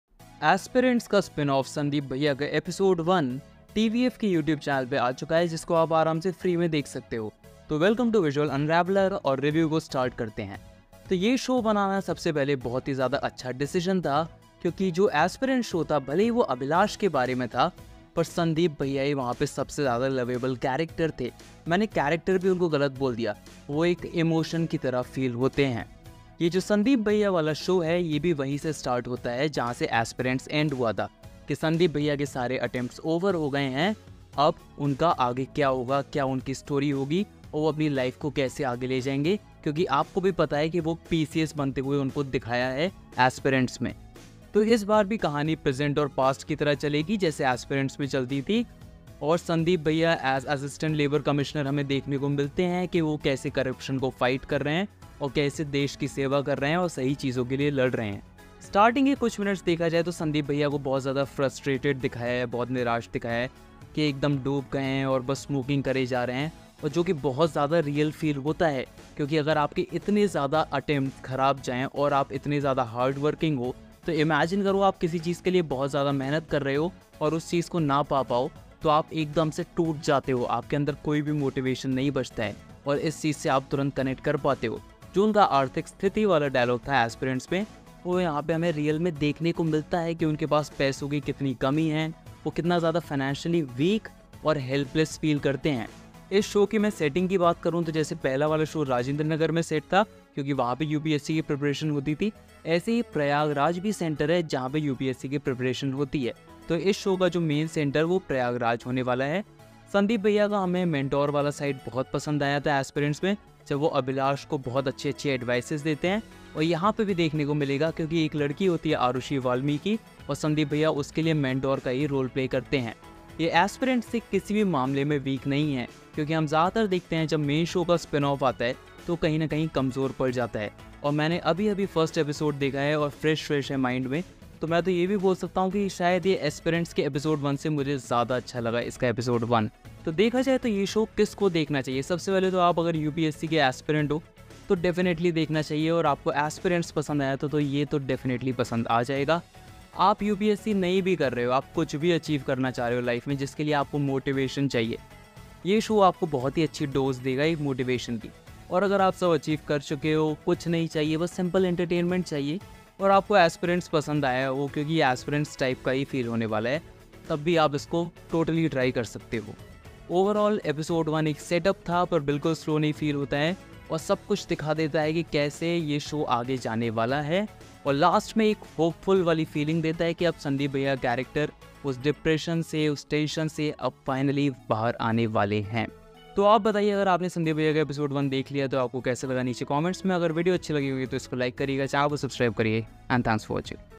Sandeep Bhaiya Review | Ep 01 | Mulyankan
Let's do the review of TVF's Sandeep Bhaiya, the spinoff or Aspirants.